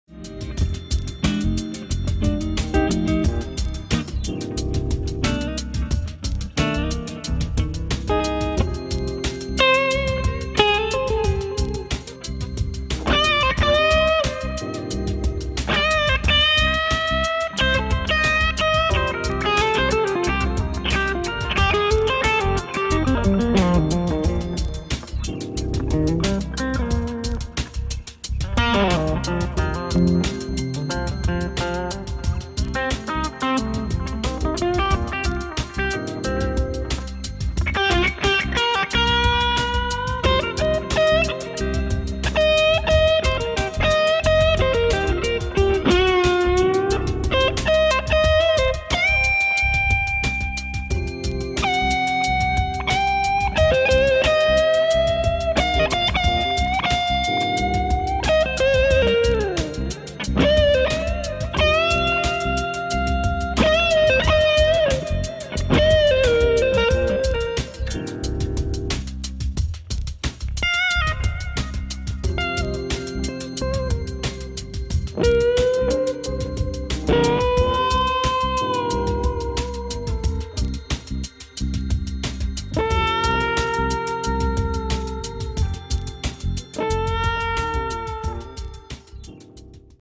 Reverend "Manta Ray" Flametop, Part2
clean first / later with Okko Diablo Gain+ Overdrive /  Amp: Fender Vibroverb (original 1963), Vahlbruch Spacetime Delay, Korg Pitchblack Tuner. Dunlop Delrin 2,0 mm picks